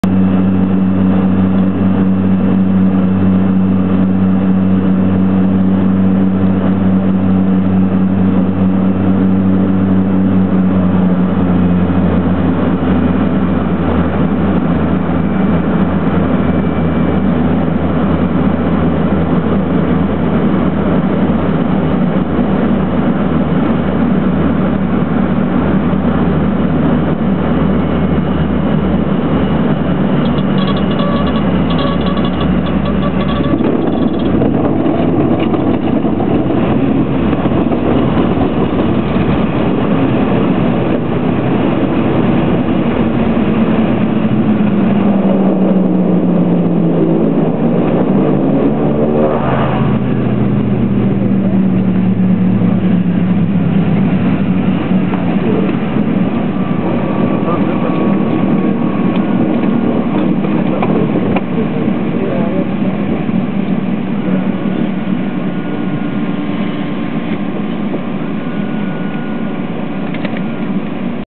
Plane Landing on Barra
The Loganair plane lands on the beach on the Isle of Barra, Outer Hebrides, Scotland.  Taken from inside the plane.